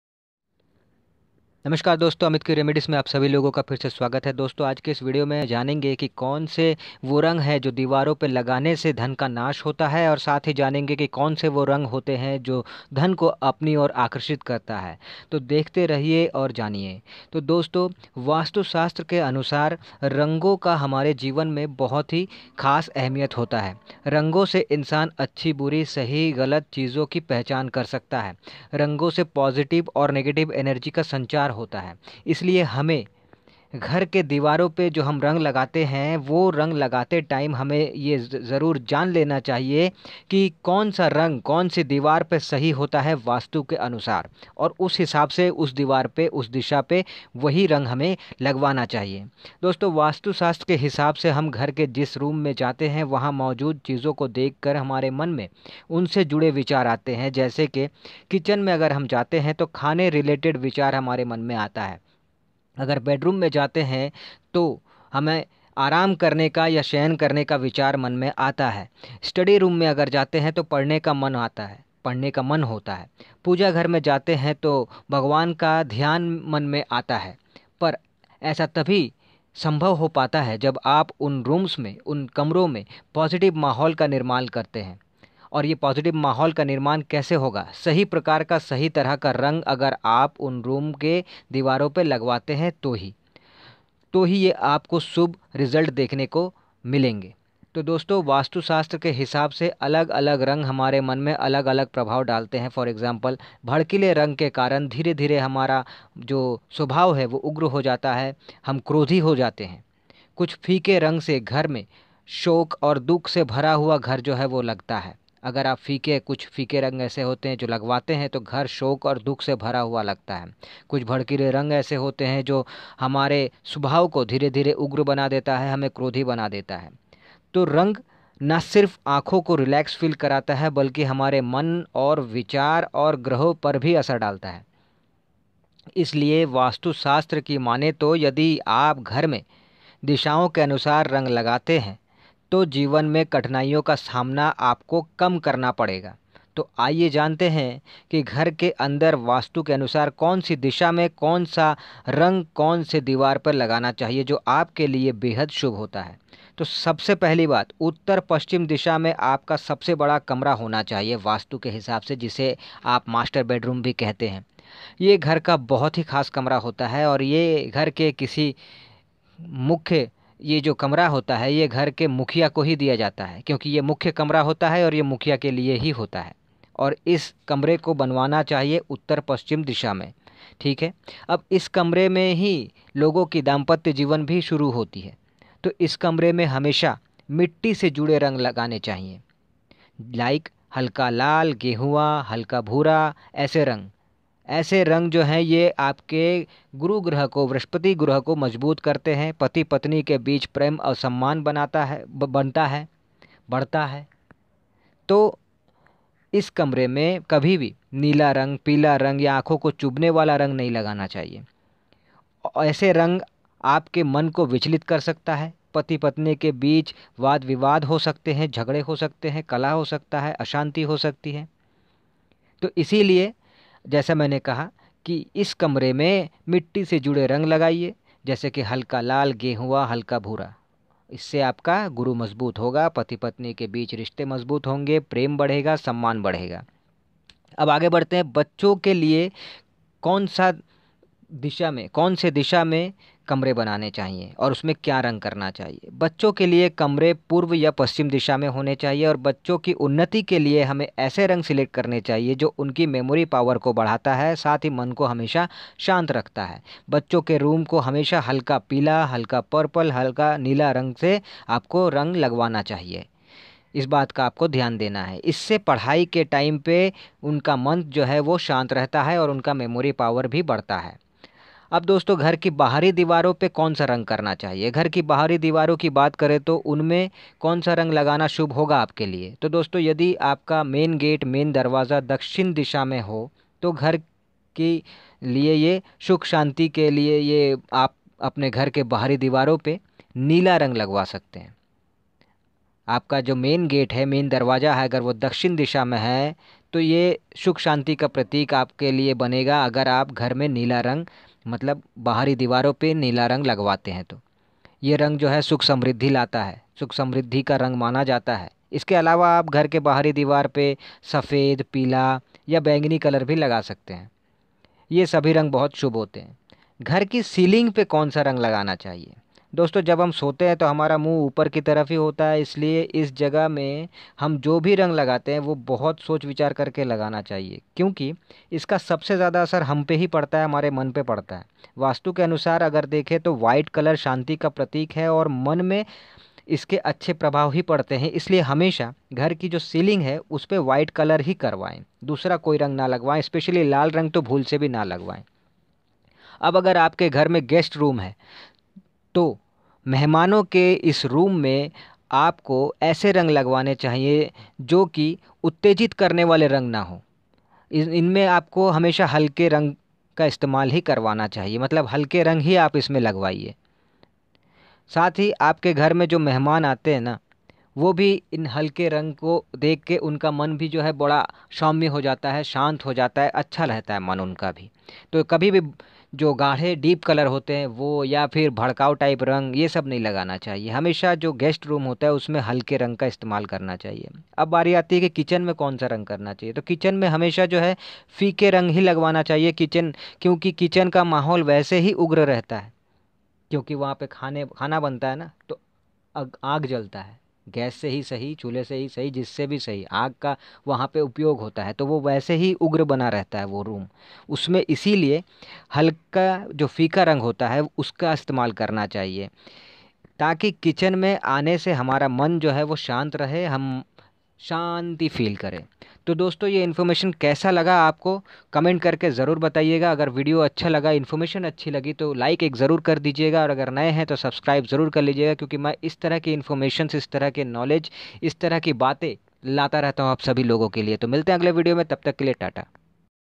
घर के दिवारो पे दिशा के अनुसार कौन सा रंग लगवाना चाहिए और कौन से नही जानिए ।
iss video mein janiye ki gahr ke kaun se disha ke diwar pe kaun sa rang lagwana chahiye taki dhan ka laabh ho har chiz ka laabh ho nuksan nhi...